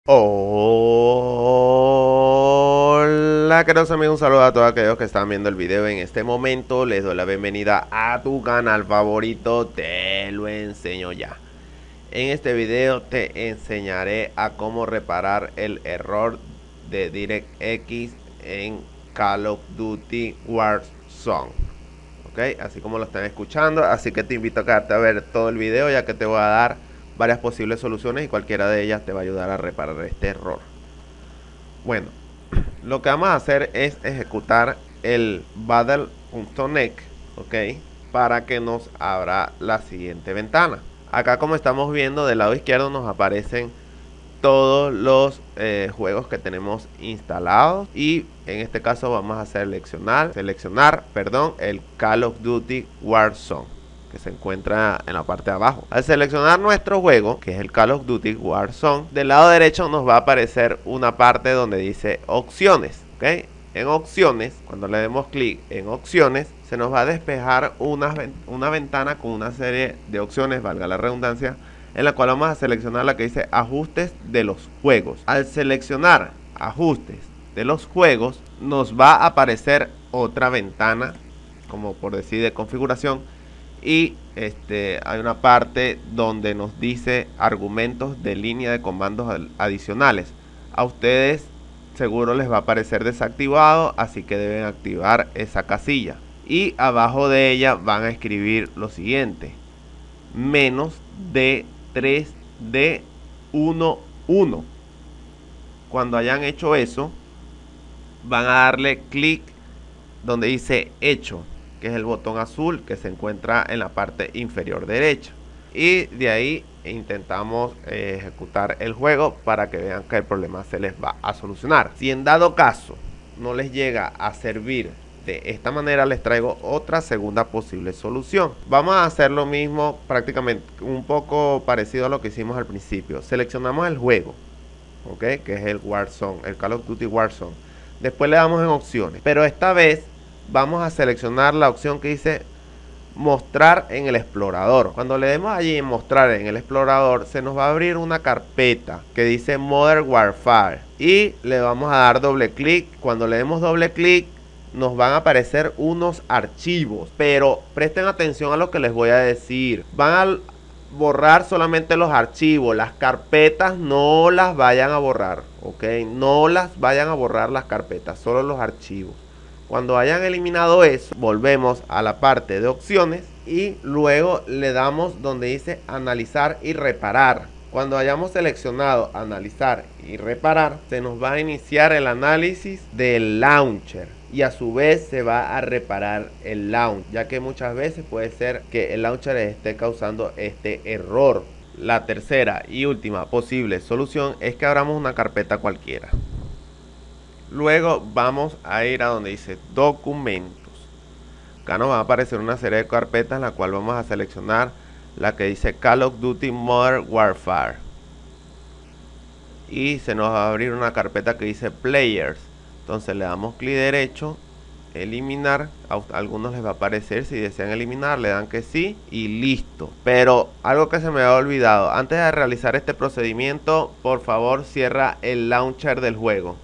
Error de DirectX en Call Of Duty Warzone en Windows 10/8/7 I 3 Soluciones 2025
En este vídeo te ayudare a Como reparar Error de DirectX en Call Of Duty Warzone en Windows 7/8/10 de una manera rápida y sencilla por medio de 3 soluciones que cualquier persona puede realizar

Videos Recomendados